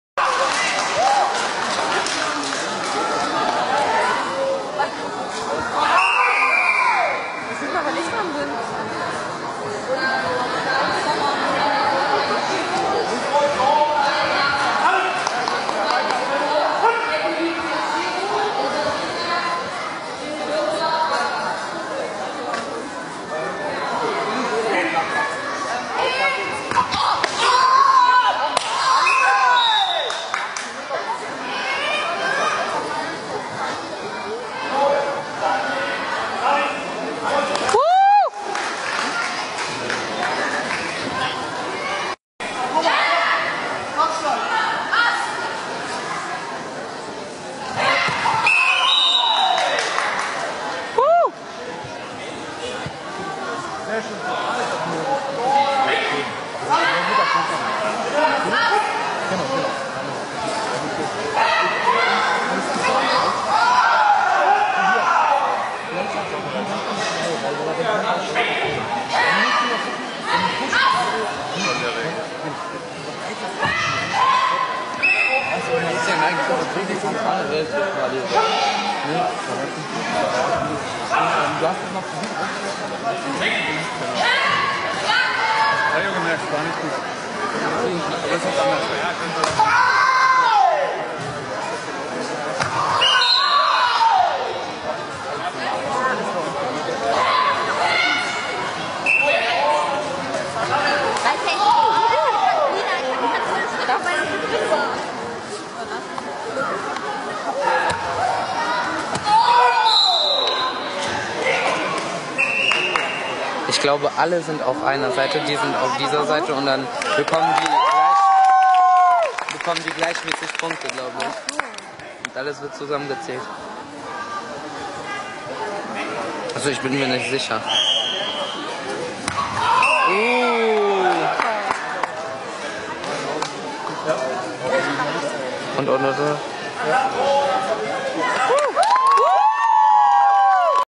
06.April 2019 Schwerin germany 18.Ostdeutschen meisterschaften des Deutschen JKA-Karate Bundes
لەپاڵەوانێتی ئەڵمانیا